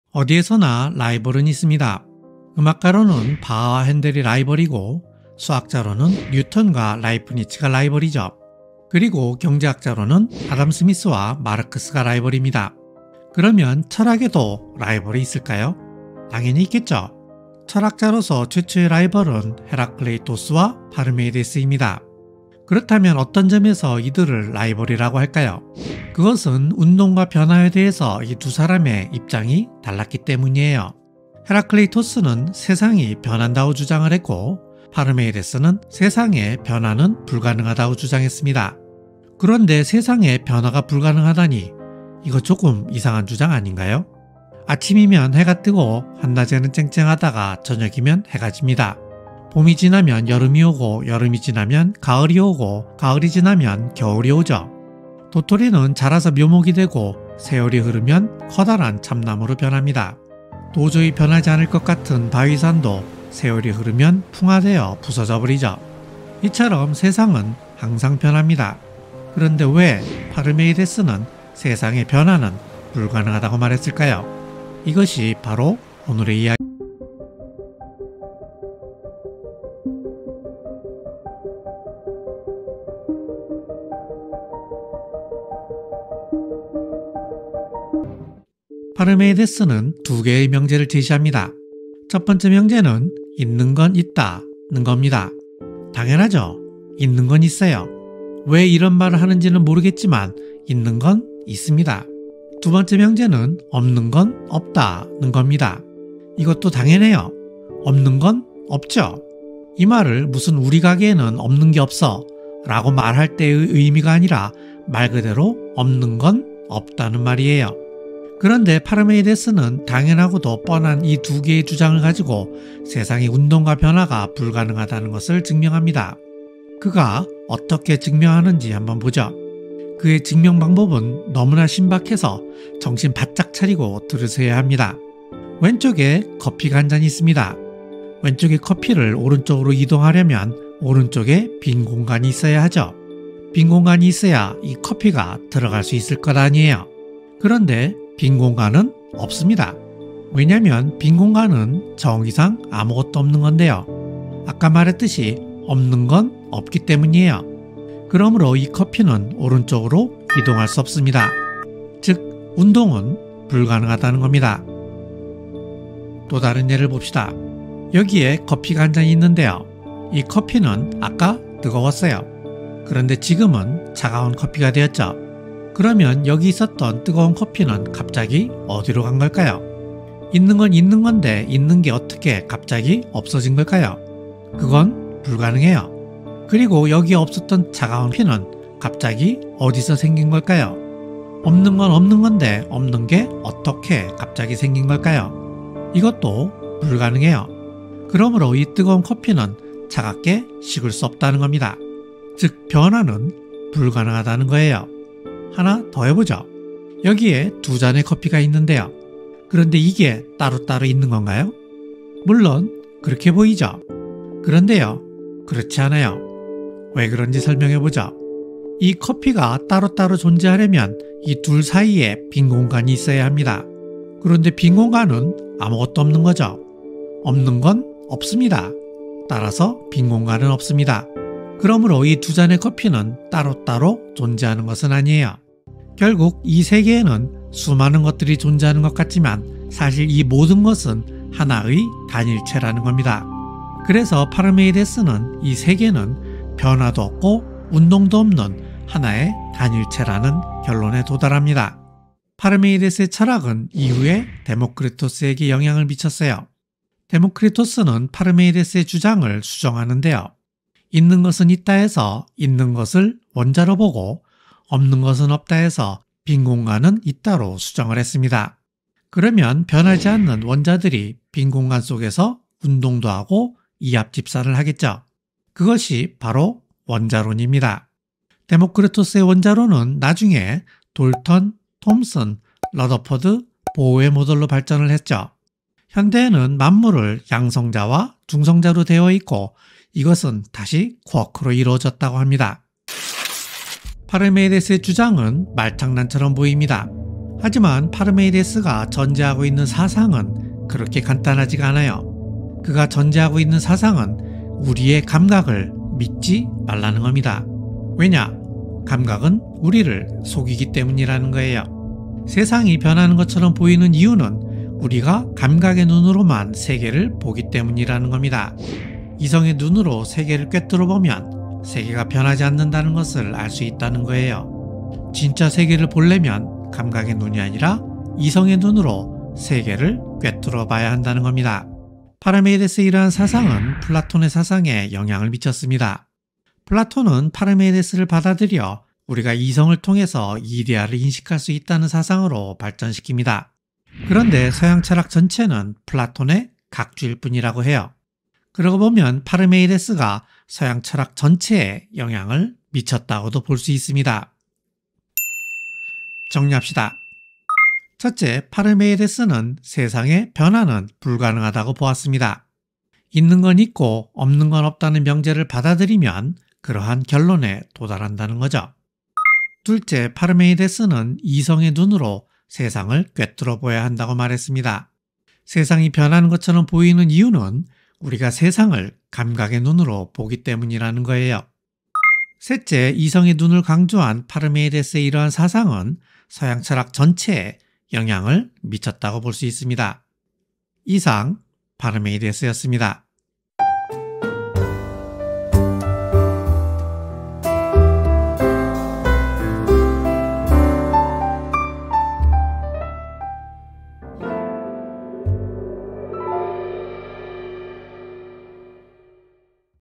006 파르메니데스: 있는 건 있고 없는 건 없다
어디에서나 라이벌은 있습니다. 음악가로는 바하와 헨델이 라이벌이고, 수학자로서는 뉴턴과 라이프니츠가 라이벌이죠. 그리고 경제학자로는 아담 스미스와 마르크스가 라이벌입니다. 그러면 철학에도 라이벌이 있을까요? 당연히 있겠죠. 철학자로서 최초의 라이벌은 헤라클레이토스와 파르메니데스입니다. 그렇다면 어떤 점에서 이들을 라이벌이라고 할까요? 그것은 운동과 변화에 대해서 이 두 사람의 입장이 달랐기 때문이에요. 헤라클레이토스 세상이 변한다고 주장했고, 파르메니데스는 세상에 변화는 불가능하다고 주장했습니다.
그런데 세상에 변화가 불가능하다니. 이거 조금 이상한 주장 아닌가요? 아침이면 해가 뜨고, 한낮에는 쨍쨍하다가, 저녁이면 해가 집니다. 봄이 지나면 여름이 오고, 여름이 지나면 가을이 오고, 가을이 지나면 겨울이 오죠. 도토리는 자라서 묘목이 되고 세월이 흐르면 커다란 참나무로 변합니다. 도저히 변하지 않을 것 같은 바위산도 세월이 흐르면 풍화되어 부서져 버리죠. 이처럼 세상은 항상 변합니다. 그런데 왜 파르메니데스는 세상에 변화는 불가능하다고 말했을까요? 이것이 바로 오늘의 이야기입니다.

* 영상제작에 참여하신 분
영상 시나리오 : 필로에듀 대표 김필영
영상 편집 : JS코믹스 대표 김주성
이미지 제작 (미드저니) : 청강문화산업대학교 교수 전혜정